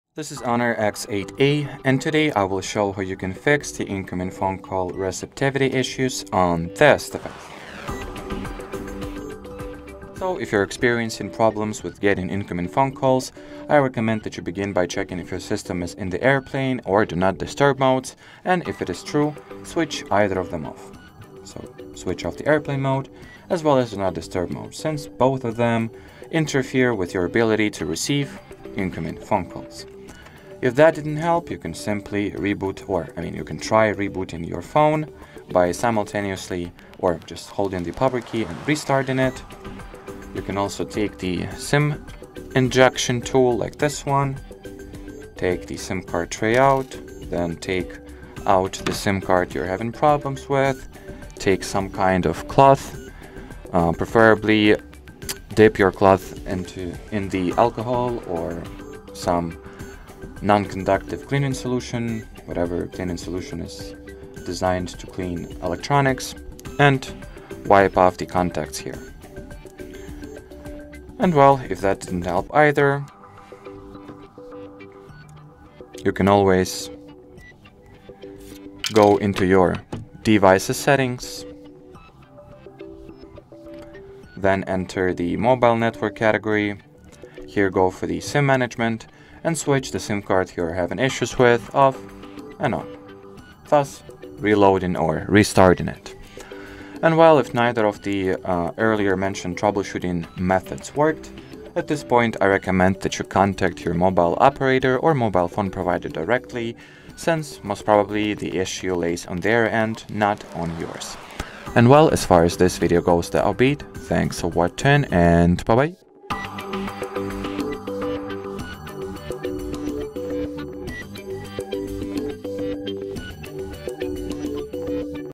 Fix Incoming Phone Call Issues On HONOR X8A
Are you experiencing issues with incoming phone calls on your HONOR X8A? Don't worry; we've got you covered. In this tutorial, we'll troubleshoot common problems like missed calls, call drops, or issues with call notifications. Learn how to check your network connection, adjust call settings, and ensure that your device's software is up to date. Follow our step-by-step guide to diagnose and fix incoming call issues on your HONOR X8A, so you never miss an important call again.

Why am I not receiving incoming calls on my HONOR X8A?
How do I check if my device's network connection is causing call issues?
Can call-blocking apps or settings affect incoming calls on the HONOR X8A?
What should I do if incoming calls are going straight to voicemail?
Is there a way to reset call settings on the HONOR X8A to default?